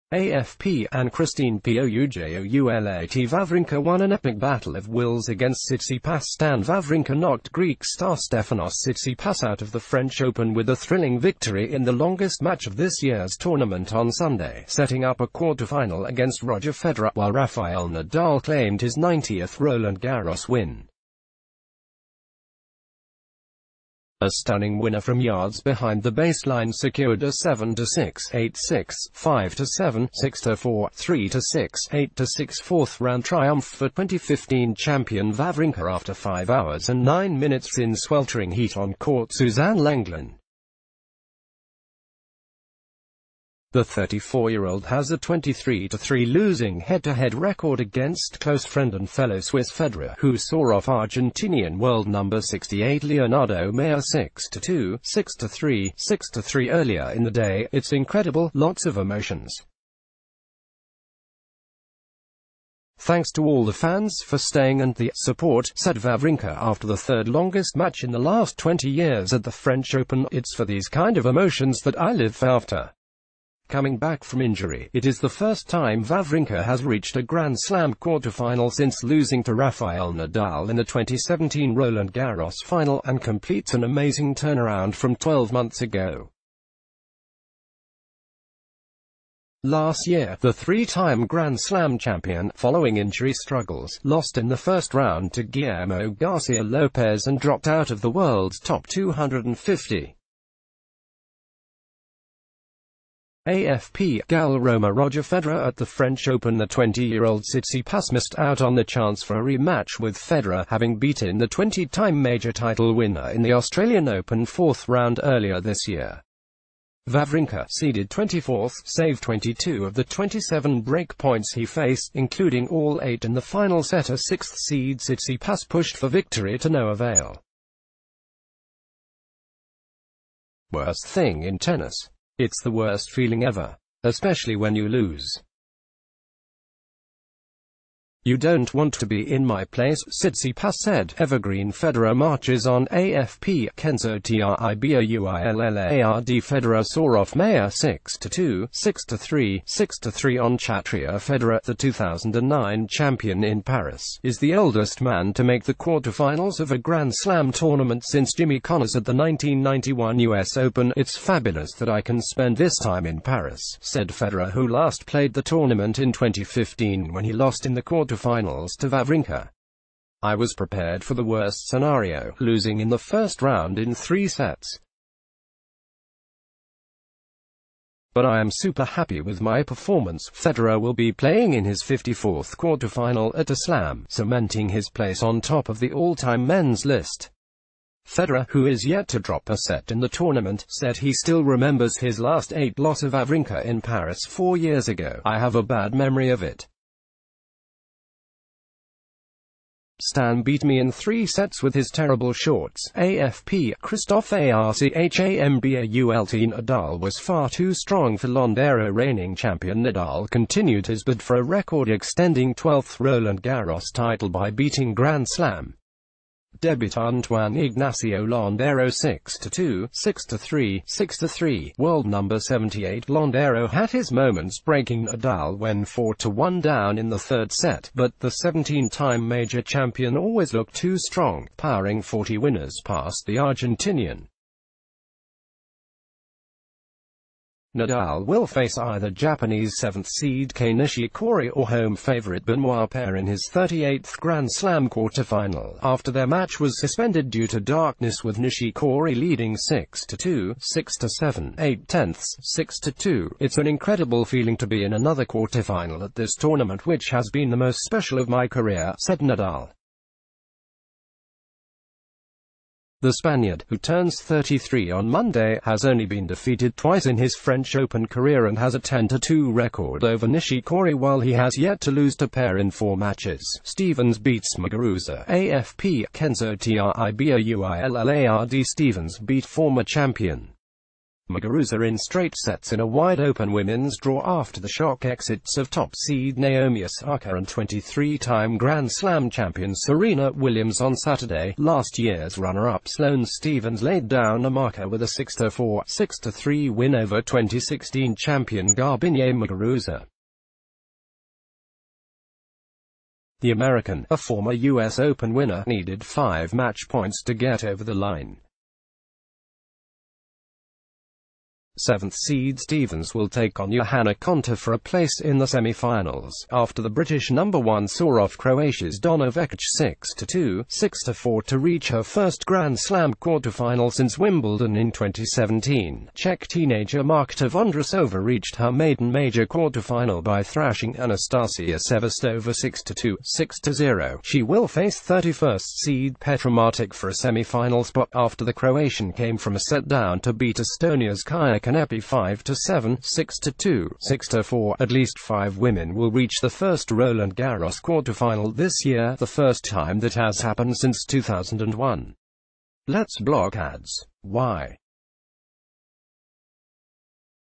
Sport News| Wawrinka sets up Federer clash after Tsitsipas epic, Nadal racks up 90th Roland Garro...
Sport News| Wawrinka sets up Federer clash after Tsitsipas epic, Nadal racks up 90th Roland Garros win